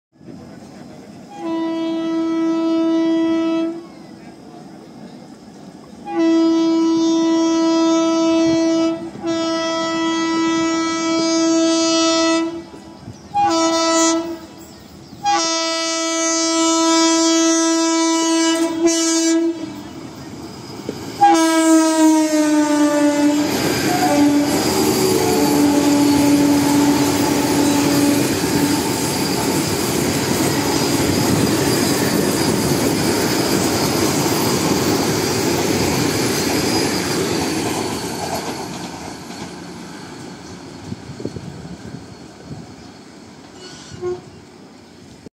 Vande Bharat Express Overtakes Jammu Mail At Kathua !! Indian Traveller !!
Vande Bharat Express Overtakes Jammu Mail At Kathua
Indian Fastest Train - Train 18 ( Vande Bharat Express )
Running Towards New Delhi